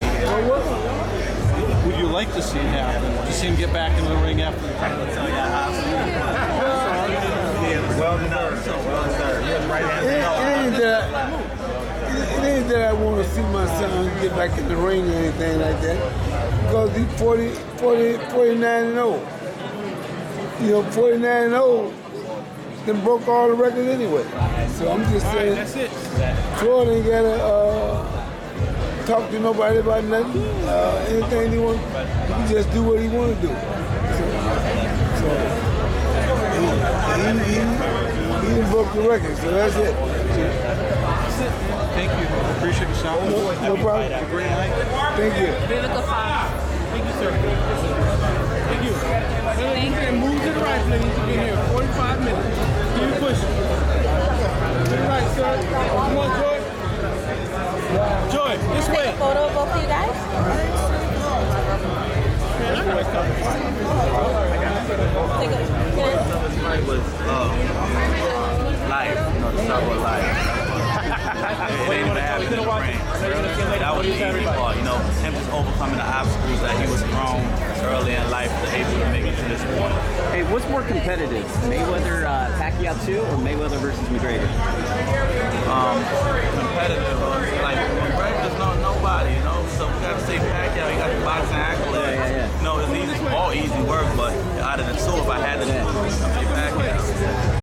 Floyd Mayweather Sr On Conor McGregor Hands Speed and floyd mayweather EsNews Boxing
In this video we interview floyd mayweather sr